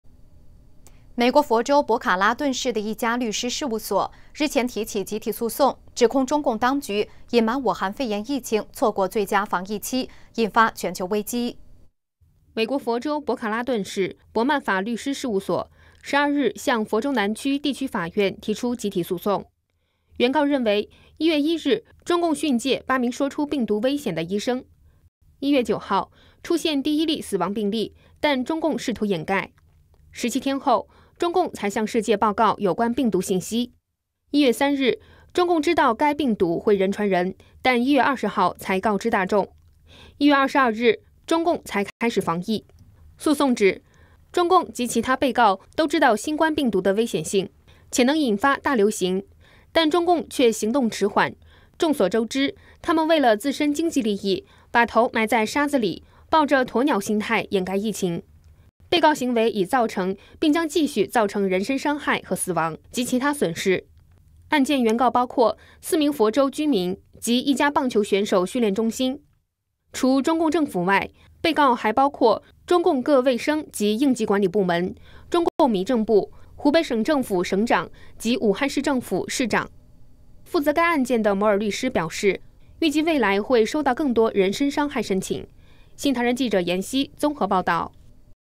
美国律师集体起诉中共：掩盖疫情危害全球 720P
文章标题：美国律师集体起诉中共：掩盖疫情危害全球

【新唐人北京时间2020年03月18日讯】美国佛州博卡拉顿市的一家律师事务所，日前提起集体诉讼，指控中共当局隐瞒武汉肺炎疫情，错过最佳防疫期，引发全球危机。

美国佛州，博卡拉顿市，柏曼法律事务所，12日向佛州南区地方法院提出集体诉讼。

原告认为：

1、1月1号，中共训诫八名说出病毒危险的医生。
2、1月9号，出现第一例死亡病例，但中共试图掩盖。
3、17天后，中共才向世界报告有关病毒信息。
4、1月3号，中共知道该病毒会人传人，但1月20号才告知大众。
5、1月22日，中共才开始防疫。

诉状指：“中国(中共)及其他被告都知道新冠病毒的危险性，且能引发大流行，但(中共)却行动迟缓，众所周知，他们为了自身经济利益，把头埋在沙子里（抱持着驼鸟心态）掩盖疫情。”

“被告行为已造成，并将继续造成人身伤害和死亡，及其他损失。”

该案原告包括，4名佛州居民及一家棒球选手训练中心。

除中共政府外，被告还包括中共各个卫生及应急管理部门、中共民政部、湖北省政府、省长及武汉市政府、市长。

负责该案的摩尔律师表示，预计未来将会收到更多“人身伤害”申诉。

新唐人记者严曦综合报导

然后人数。

大纪元

2005年1月12日